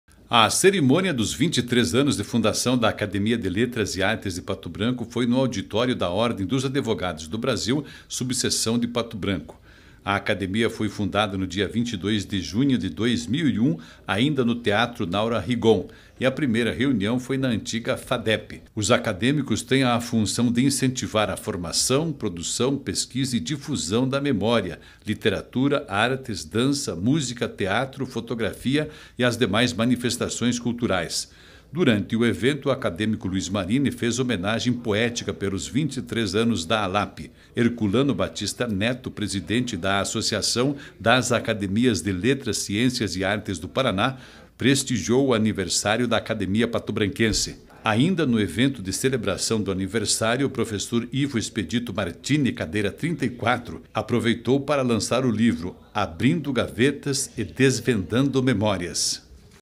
Academia de Letras e Artes de Pato Branco celebra seus 23 nos de existência
Eventos: A ALAP – Academia de Letras e Artes de Pato Branco, celebra seus 23 anos de história. Durante a cerimônia, integrante da academia lançou seu livro.